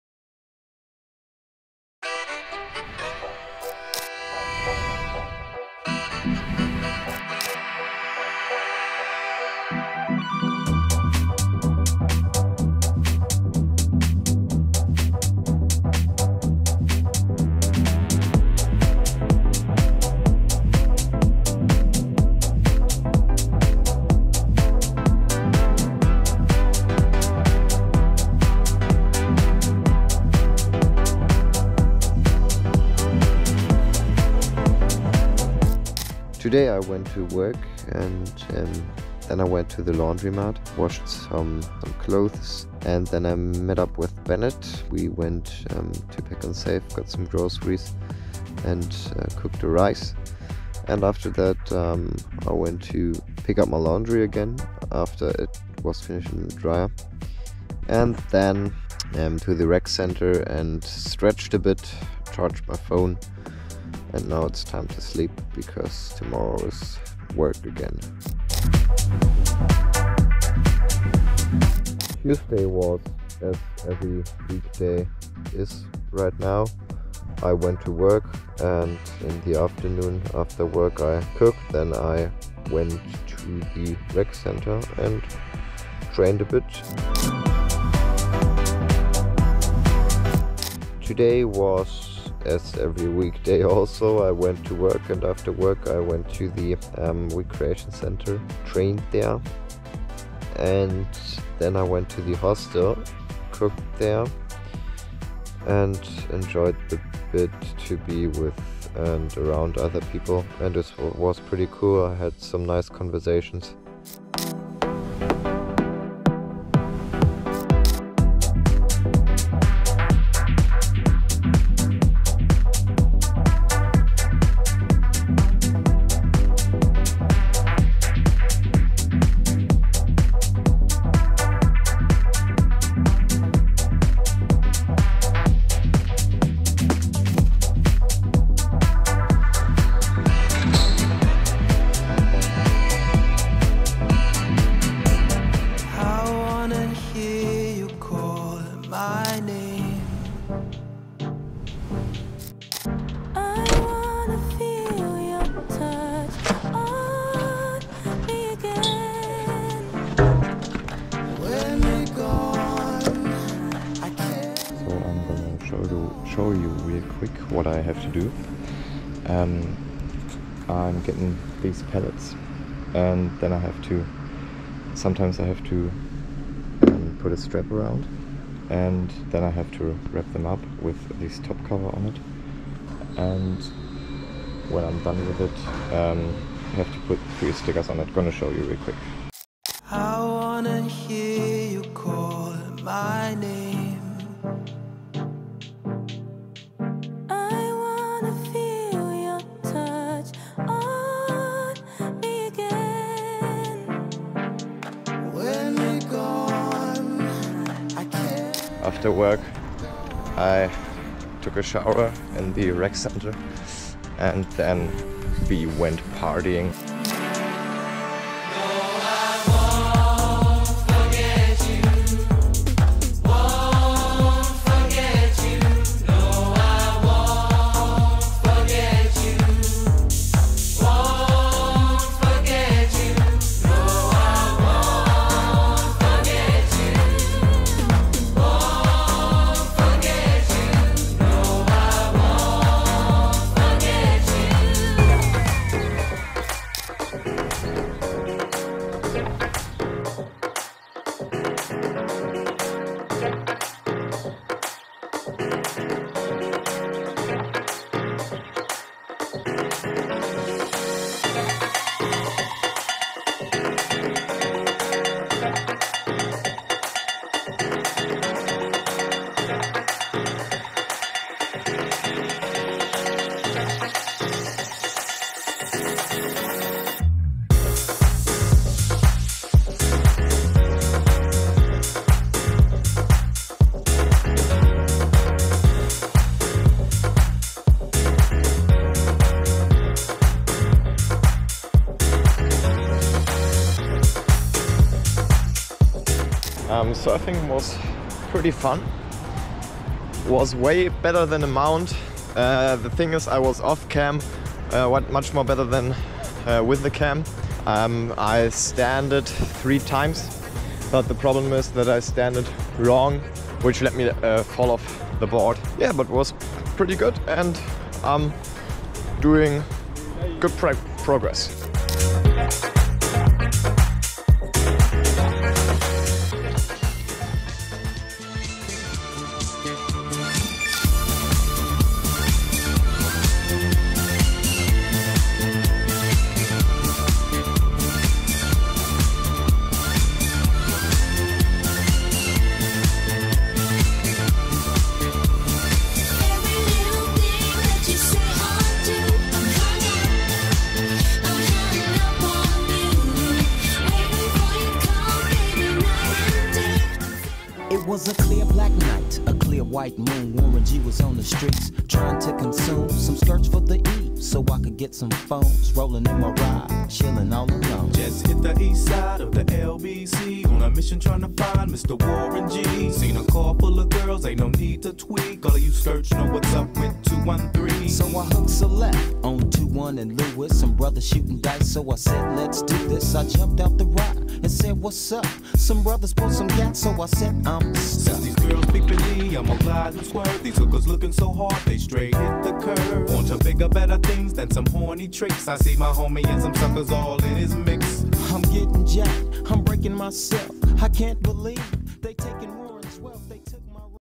WEEK15 - Went surfing again! | Work And Travel In New Zealand
Hopefully I’m gonna get a video uploaded every week.

And please tell me, if the color-grading is good.

Chapters:
00:00 Monday 26.02.24
01:09 Tuesday 27.02.24
01:28 Wednesday 28.02.24
01:55 Thursday 29.03.24
02:30 Friday 01.03.24
04:09 Saturday 02.03.24
06:00 Sunday 03.03.24

Equipment:
GoPro Hero 11 Black
iPhone 12mini
Davinchi Resolve
iPad Pro